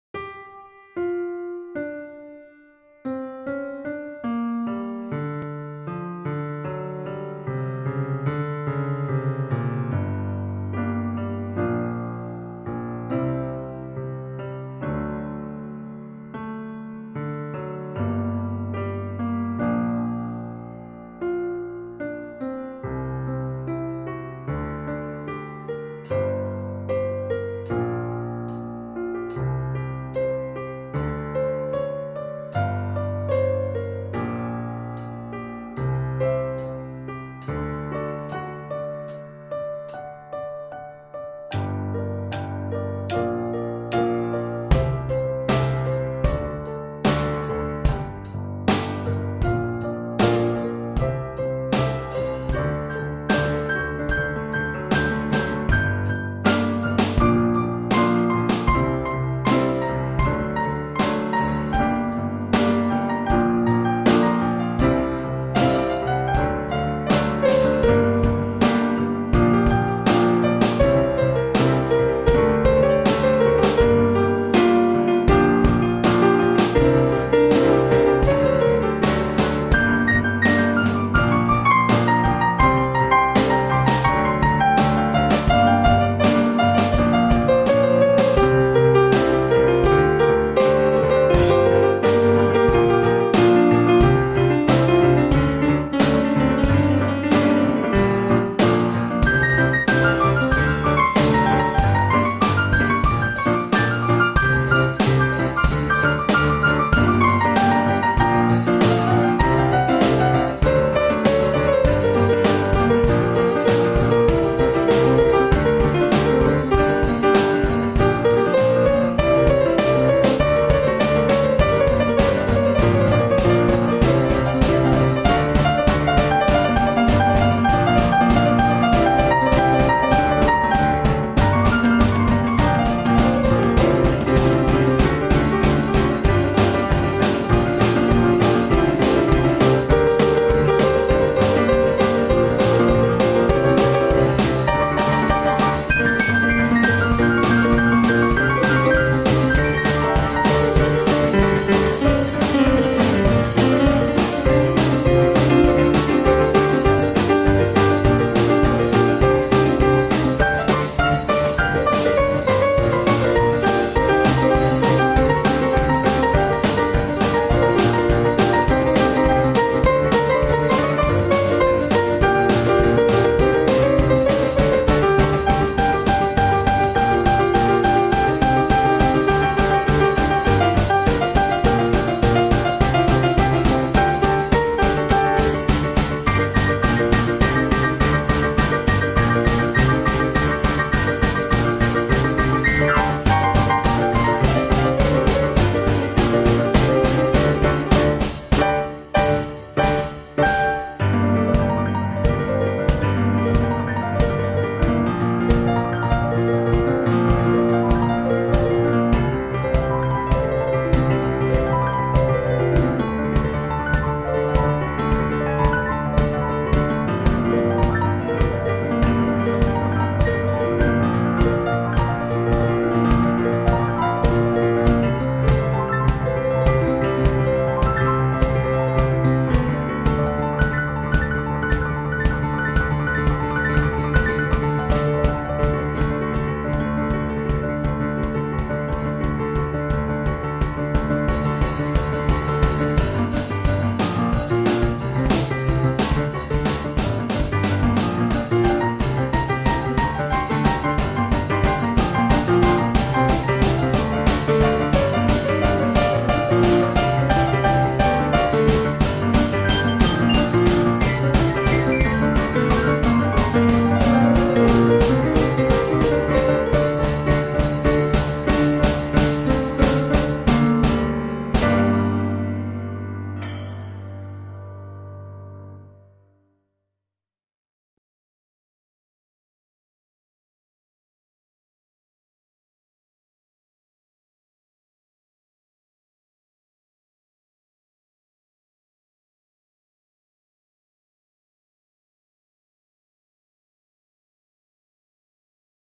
Piano improvisation
Okay, so this is a one shot improvisation, I just hit record on my software and played it only once hence the mess sometimes during the song.
Sorry for bad video quality. At least the sound is good =)